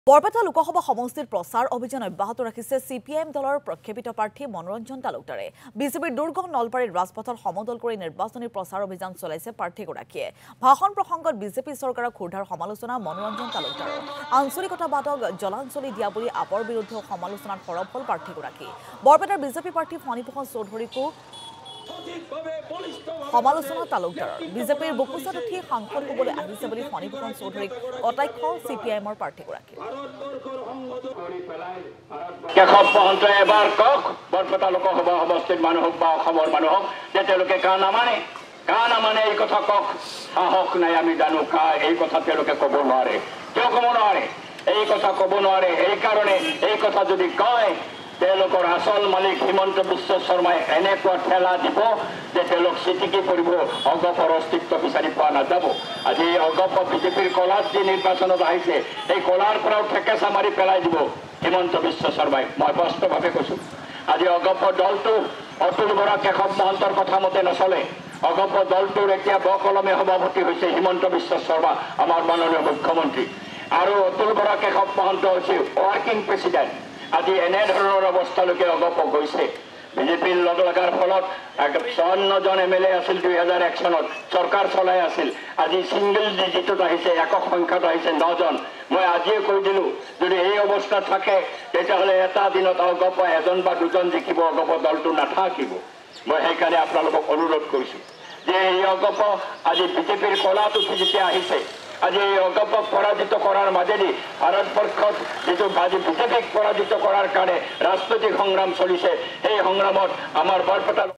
প্ৰচাৰ অভিযান অব্যাহত CPI(M)ৰ প্ৰাৰ্থী মনোৰঞ্জন তালুকদাৰৰ....
প্ৰচাৰ অভিযান অব্যাহত CPI(M)ৰ প্ৰাৰ্থী মনোৰঞ্জন তালুকদাৰৰ। নলবাৰীৰ ৰাজপথত সমদল কৰি নিৰ্বাচনী প্ৰচাৰ অভিযান। ভাষণ প্ৰসংগত বিজেপি চৰকাৰক ক্ষুৰধাৰ সমালোচনা।